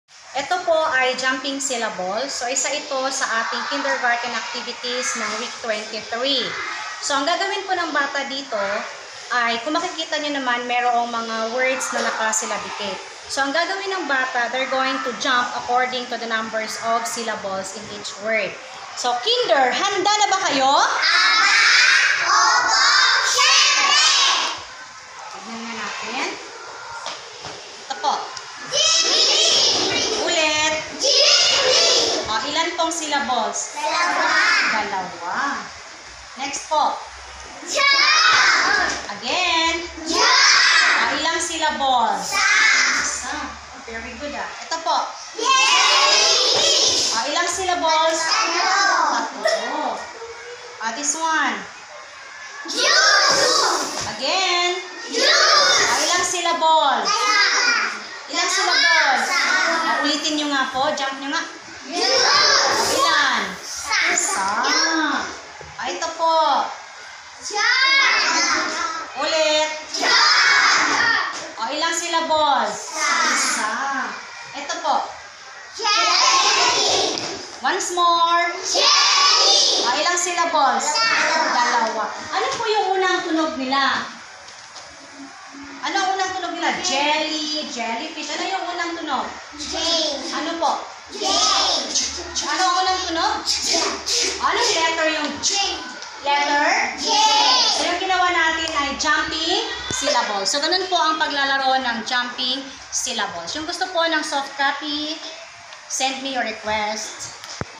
Jumping Syllables version 1/ Kindergarten Activity/ Educational Video
One of the Kindergarten's Week 23 Lessons. Please drop your comments after watching mga kaguro. Pakicomment po after watching para alam ko po na isa kayo sa nakapanood :) Thank you po :)